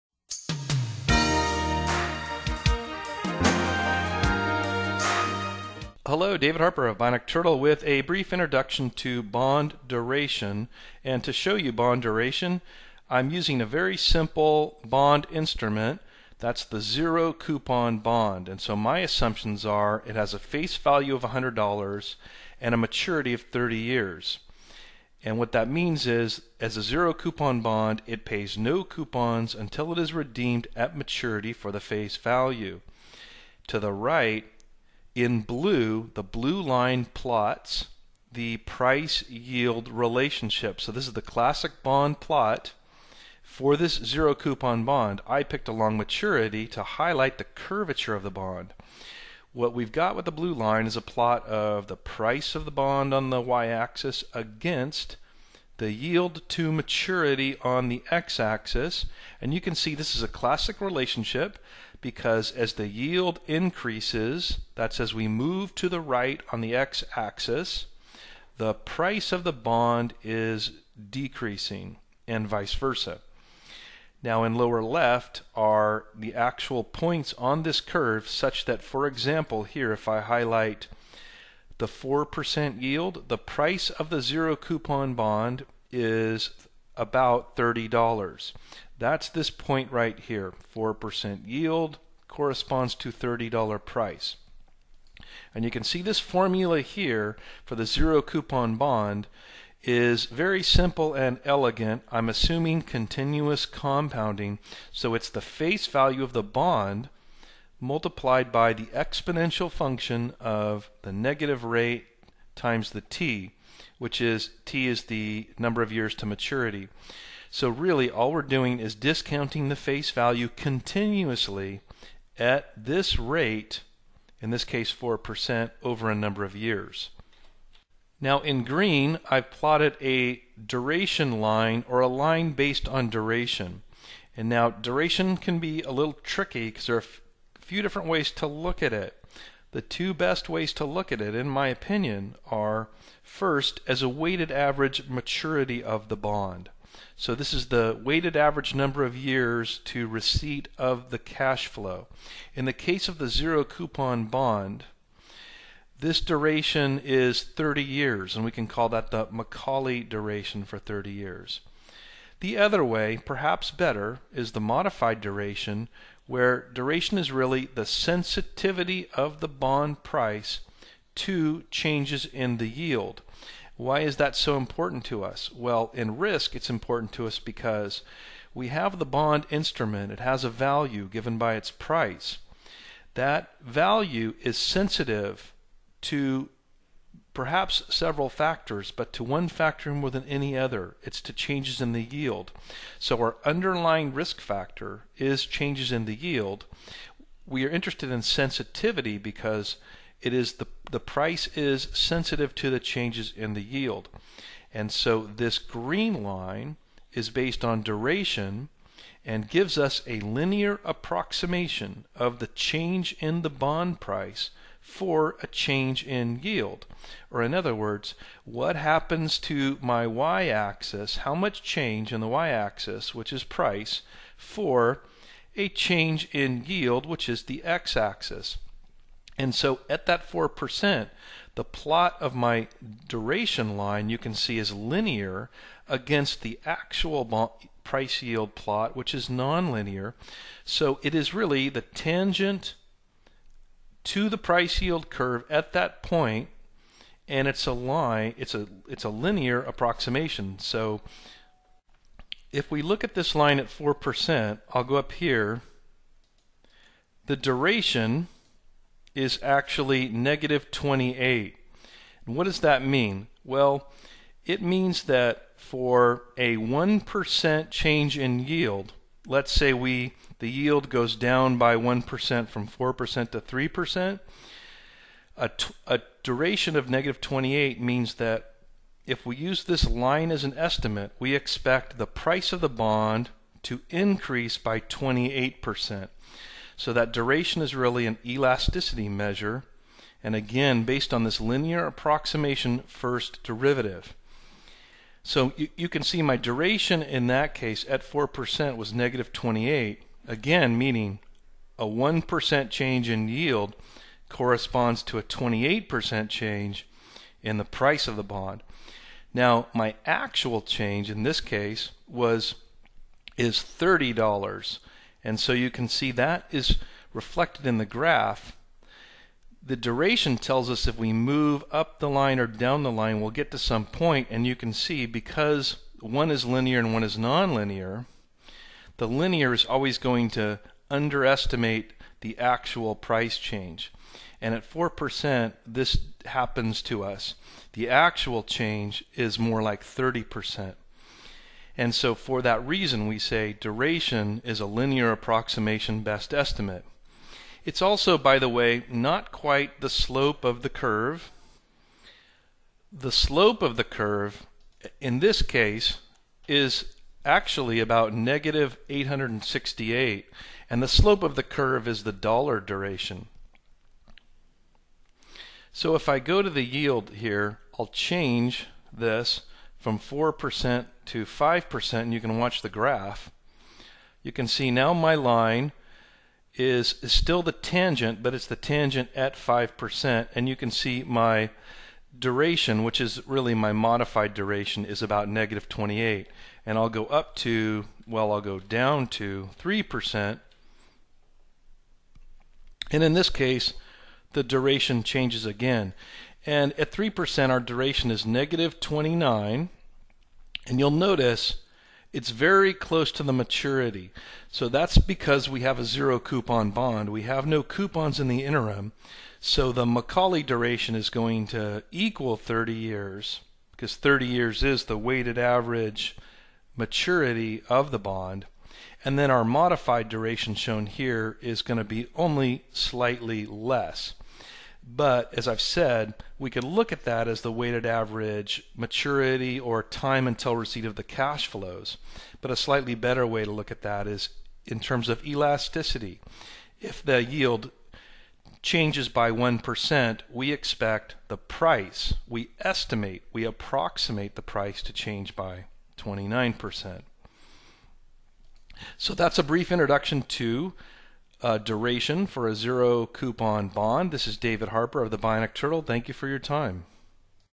FRM: Bond duration (introduction)
We have a few variations, including weighted average time to cash flow, but the best way to view duration is as a SENSITIVITY: the % change in bond price given a % change in yield (YTM).